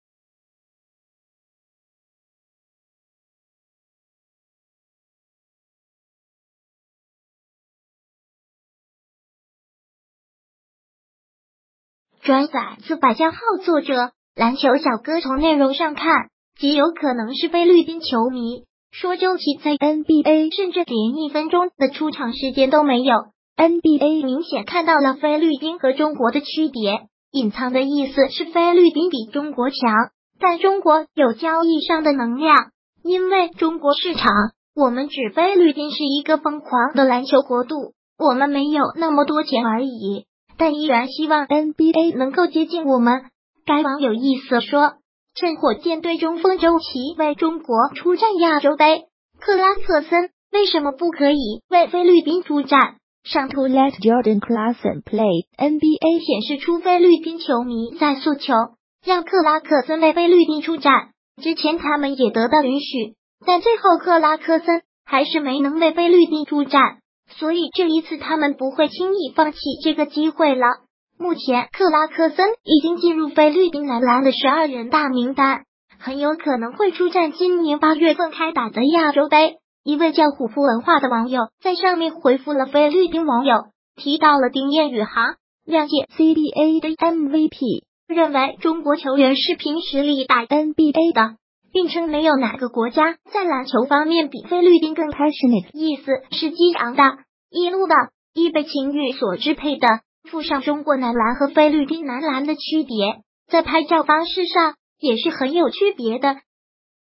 菲律宾网友喷周琦打NBA是因中国市场！认为菲律宾男篮更强！
转载自百家号作者：篮球小哥从内容上看，极有可能是菲律宾球迷。说周琦在NBA甚至连一分钟的出场时间都没有。NBA明显看到了菲律宾和中国的区别，（隐藏的意思是菲律宾比中国强）但中国有交易上的能量，因为中国市场。我们（指菲律宾）是一个疯狂的篮球国度，我们没有那么多钱而已，但依然希望NBA能够接近我们。该网友意思说，称火箭队中锋周琦为中国出战亚洲杯，克拉克森为什么不可以为菲律宾出战？上图let Jordan clarson play＃NBA显示出菲律宾球迷在诉求，让克拉克森为菲律宾出战。之前他们也得到允许，但最后克拉克森还是没能为菲律宾出战，所以这一次他们不会轻易放弃这个机会了！目前克拉克森已经进入菲律宾男篮的十二人大名单，很有可能会出战今年八月份开打的亚洲杯！一位叫虎扑文化的网友在上面回复了菲律宾网友，提到了丁彦雨航，两届CBA的MVP，认为中国球员是凭实力打NBA的，并称没有哪个国家在篮球方面比菲律宾更passionate，（意思是激昂的; 易怒的; 易被情欲所支配的）附上中国男篮和菲律宾男篮的区别，在拍照方式上也是很有区别的！